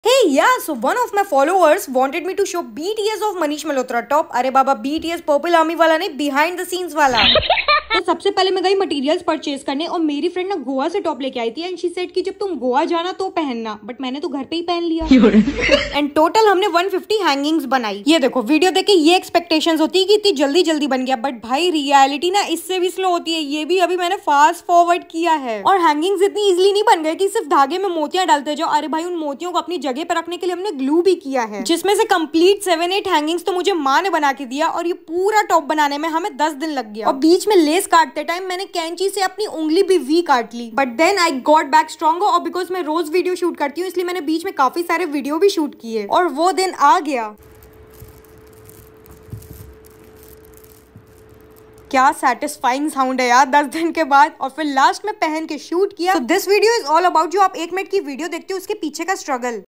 BTS of Designer Top 😱
Here is a BTS video of Designer Top that I made and what all struggle iI faced.
I'm back with fun DIY. This beautiful Top isa recreation of designer top by Manish Malhotra for Janhvi Kapoor's NMACC look and it took me 10 days to complete it and I personally enjoyed the process. Do recreate and share your creation with me on my Instagram Page and get featured.